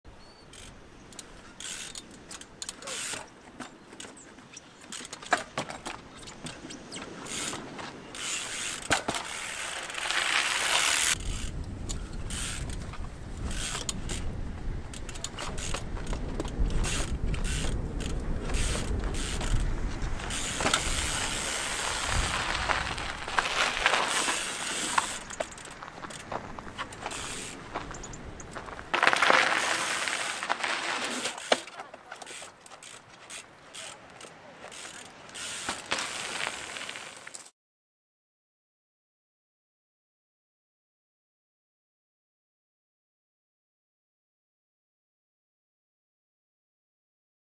Bike Trials - Trials Riding On The Rocks
A few clips of trials riding on the rocks.